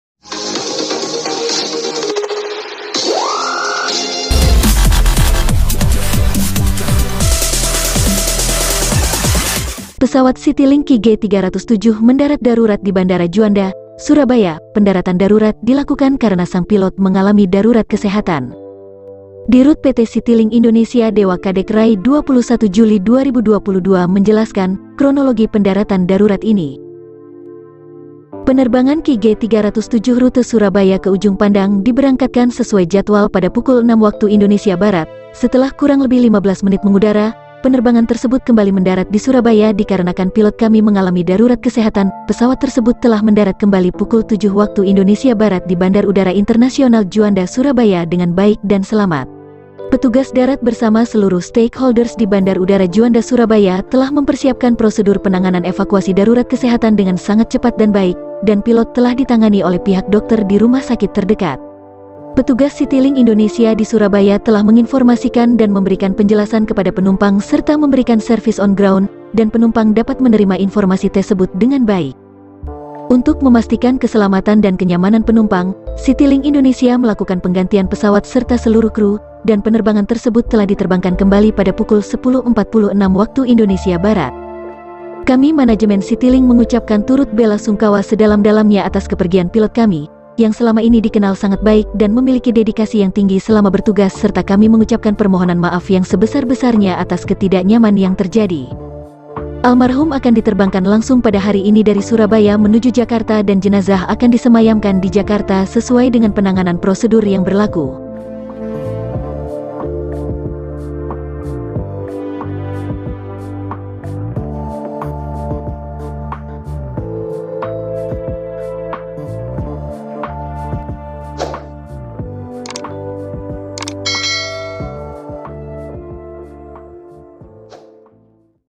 Kronologi Citilink Mendarat darutat & Pilot Meninggal dunia
Penerbangan QG307 rute Surabaya ke Ujung Pandang diberangkatkan sesuai jadwal pada pukul 06.00 WIB. Setelah kurang lebih 15 menit mengudara, penerbangan tersebut kembali mendarat di Surabaya dikarenakan pilot kami mengalami darurat kesehatan. Pesawat tersebut telah mendarat kembali pukul 07.00 WIB di Bandar Udara Internasional Juanda Surabaya dengan baik dan selamat.

Manajemen Citilink mengucapkan turut bela sungkawa sedalam-dalamnya atas kepergian pilot kami, yang selama ini dikenal sangat baik dan memiliki dedikasi yang tinggi selama bertugas serta kami mengucapkan permohonan maaf yang sebesar besarnya atas ketidaknyaman yang terjadi.

Sumber detiknews